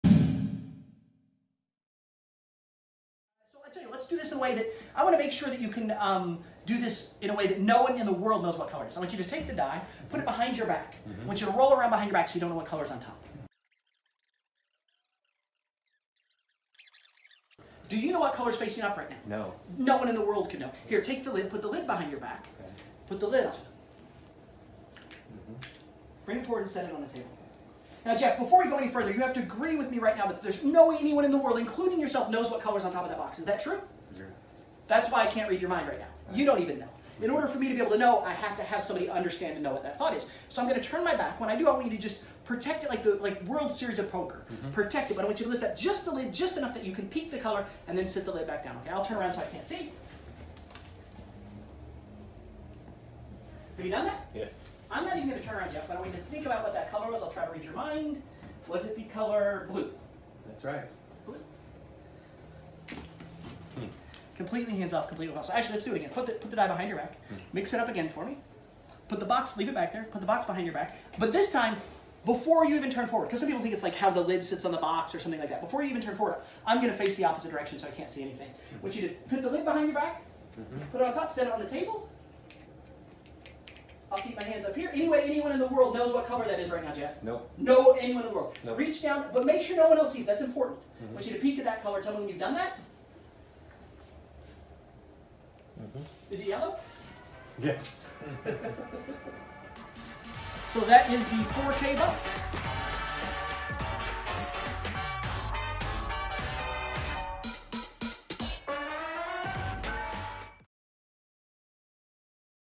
4K Color Vision Box de Magic Firm Video 2- Truco de magia 🎩
✔️Descripción:
Esta increíble puesta al día del clásico Clarividencia con Dados es multifacética y dejará a todos boquiabiertos, con un accesorio simple que puedes realizar en cualquier momento y en cualquier lugar. Video 2